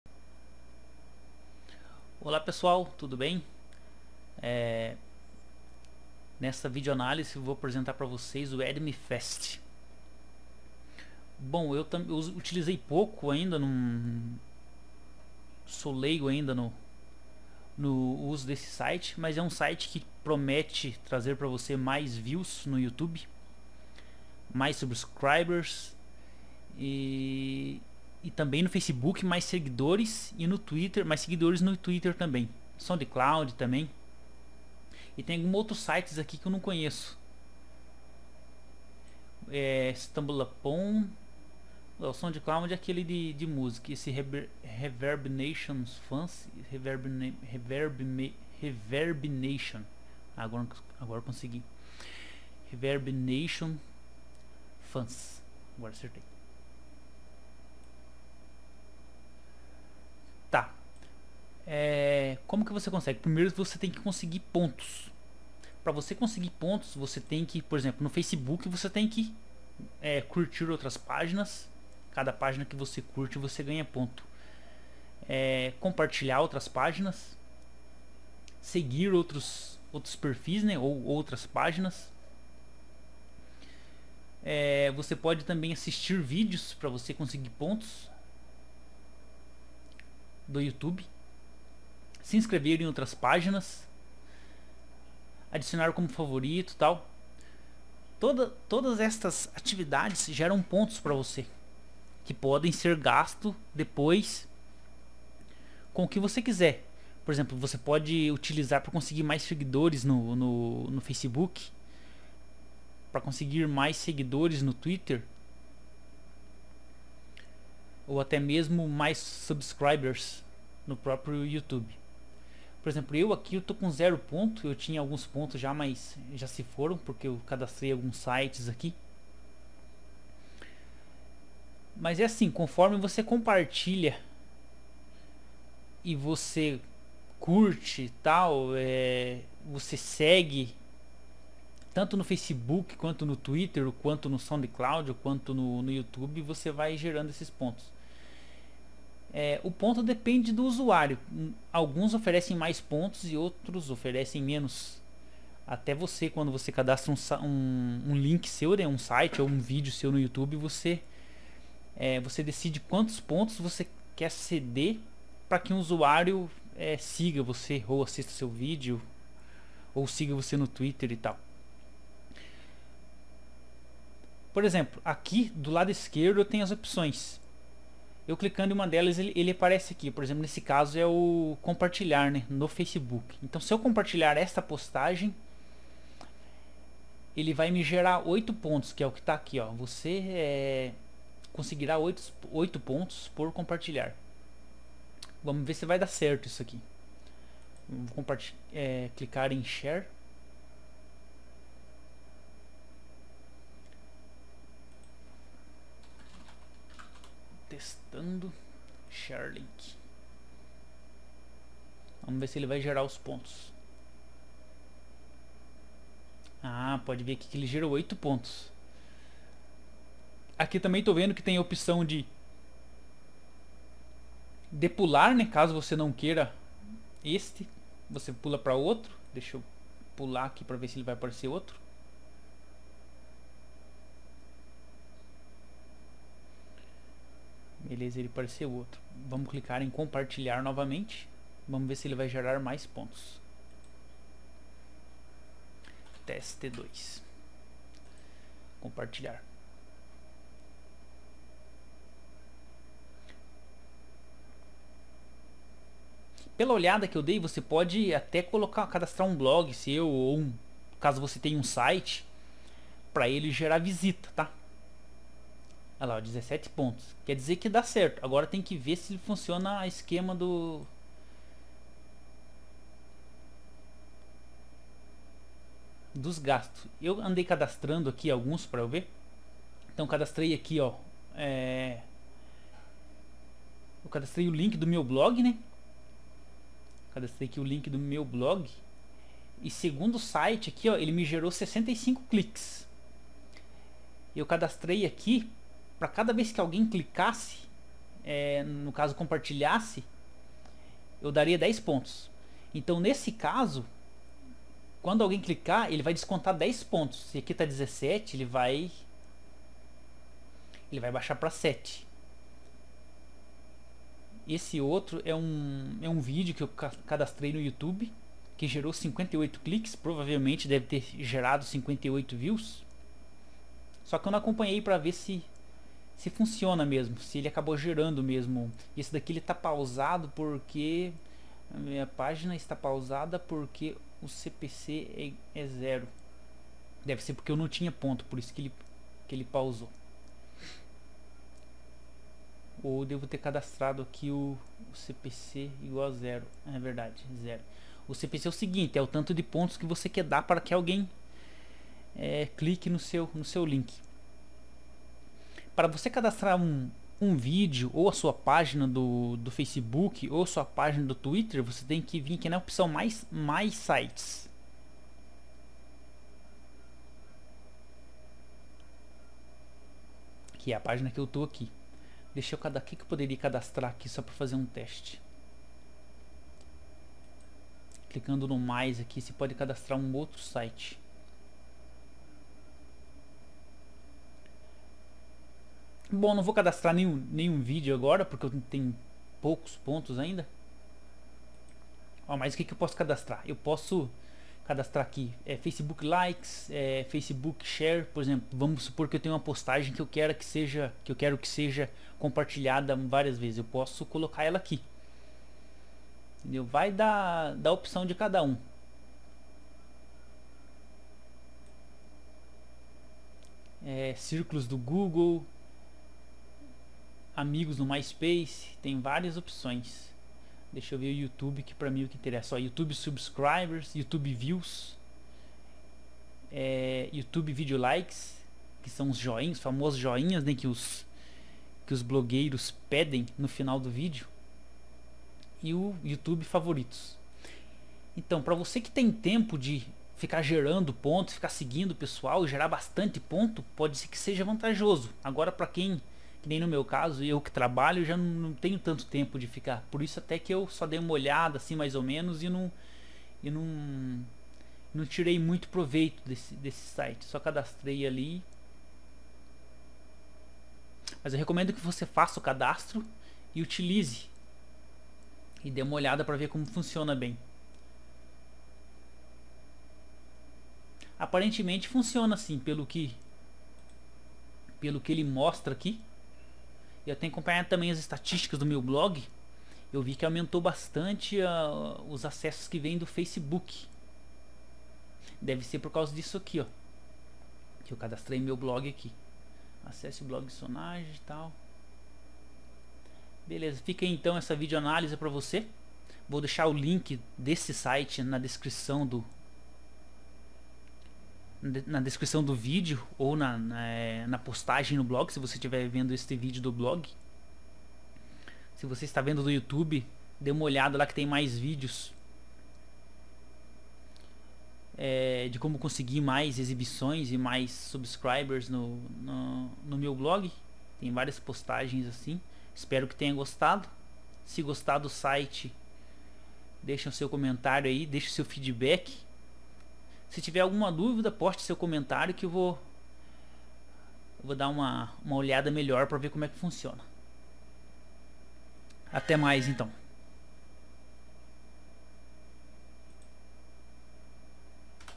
Site promete mais inscritos, views, likes e favoritos no youtube | Vídeo análise
Olá Galera, apresento-lhes o addme fast. É um site que promete mais inscritos,mais views, mais likes e favoritos no YouTube. Mas o melhor é que não é um serviço direcionado somente para o YouTube mas sim para várias outras plataformas como o Facebook (mas likes nas suas páginas, mais compartilhamentos nas suas postagens. Ideal para divulgação de blogs sites e outros links, como vídeos do youtube ou música do soundcloud), Twitter (ajuda você a conseguir mais seguidores), Soundcloud, Google mais e outras que não são tão conhecidas. Não perca tempo faça já o seu cadastro e experimente este serviço, já que você não vai gastar nada mesmo, a não ser o seu precioso tempo. Clique no link logo acima para ser redirecionado.